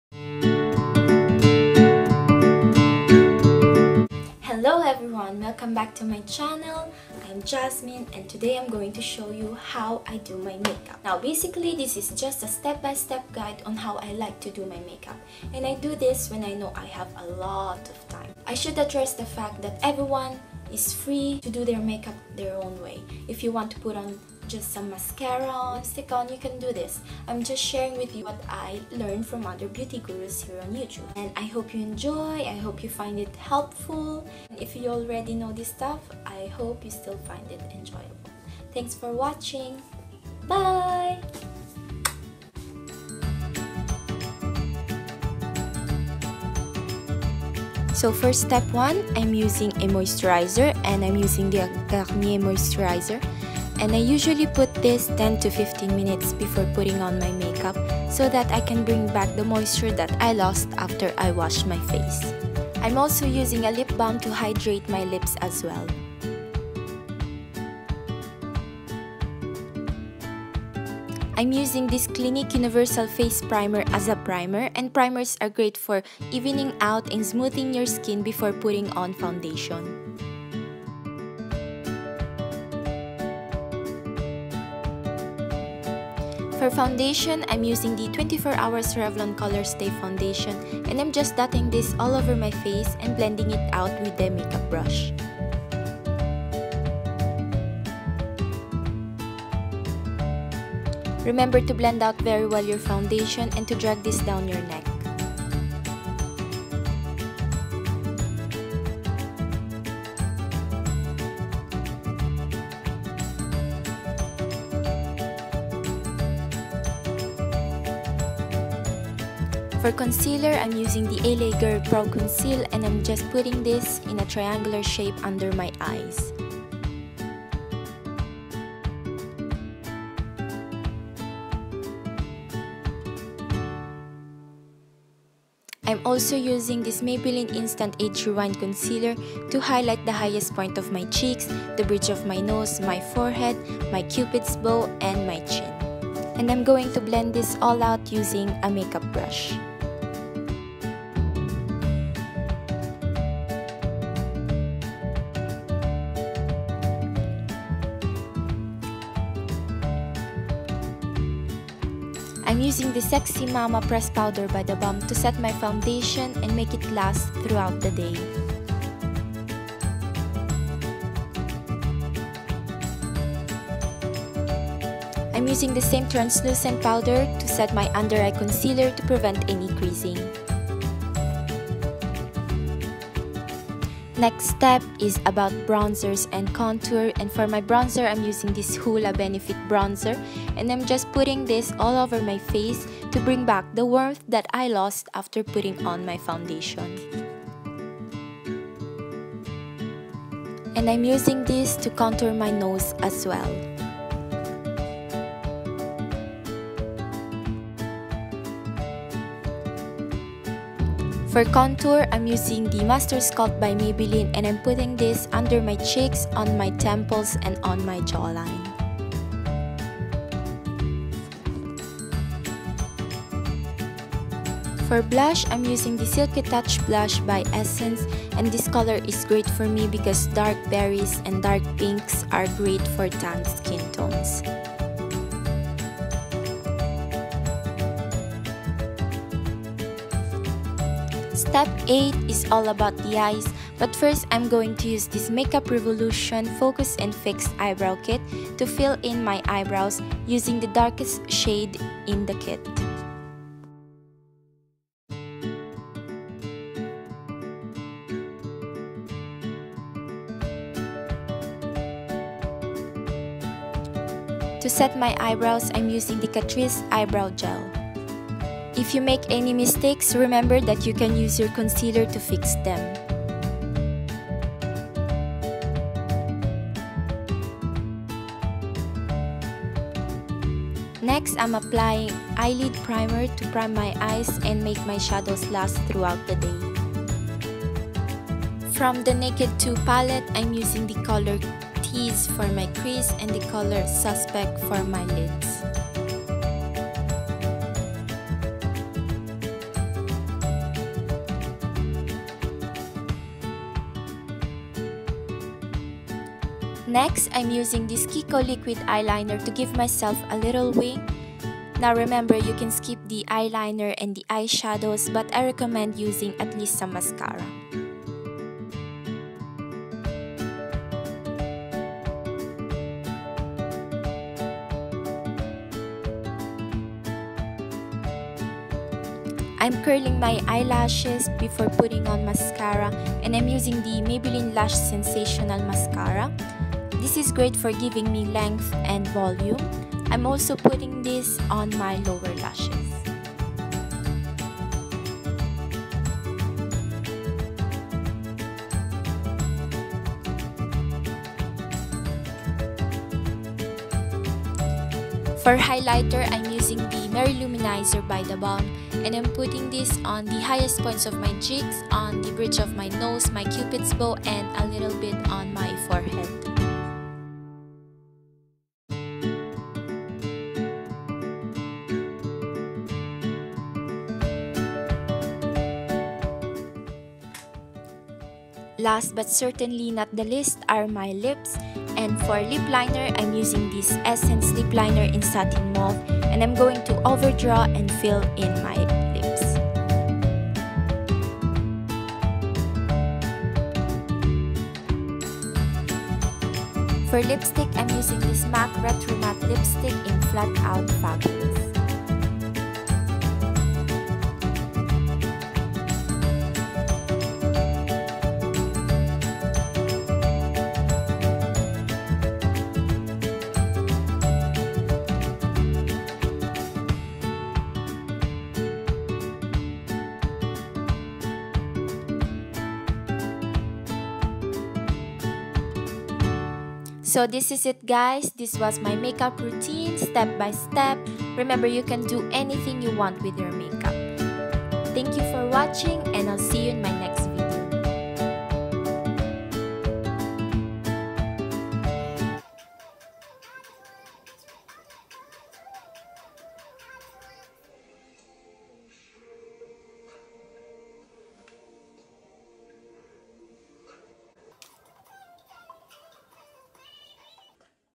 MY MAKEUP ROUTINE STEP BY STEP | TALK THROUGH
HELLO MESMERIZING BEAUTIES!
THIS IS MY MAKE UP ROUTINE STEP BY STEP. I AM SHARING WITH YOU WHAT I LEARNT SO FAR FROM OTHER BEAUTY GURUS HERE ON YOUTUBE.
REMEMBER YOU CAN DO WHATEVER YOU WANT :)

PRODUCTS MENTIONED
GARNIER MOISTURIZER
CLINIQUE UNIVERSAL FACE PRIMER
REVLON 24 HRS COLORSTAY FOUNDATION
LA GIRL PROCONCEAL CONCEALER
MAYBELLINE INSTANT AGE REWIND CONCEALER
THE BALM SEXY MAMA TRANSLUCENT POWDER
BENEFIT HOOLA BRONZER
MAYBELLINE MASTERSCULPT CONTOUR KIT
MAKEUP REVOLUTION EYEBROW SHAPE KIT
CATRICE EYEBROW GEL
URBAN DECAY NAKED 2 PALETTE ( TEASE , SUSPECT)
KIKO DEFINITION LIQUID EYELINER
MAYBELLINE LASH SENSATIONAL MASCARA
THE BALM MARY-LOU MANIZER HIGHLIGHTER
ESSENCE LIPLINER (SATIN MAUVE)
MAC LIPSTICK RETROMATTE ( FLAT OUT FABULOUS)

PS. IM SORRY FOR THE LAST PART, THERE WAS SOMETHING WRONG WITH MY MIC.:)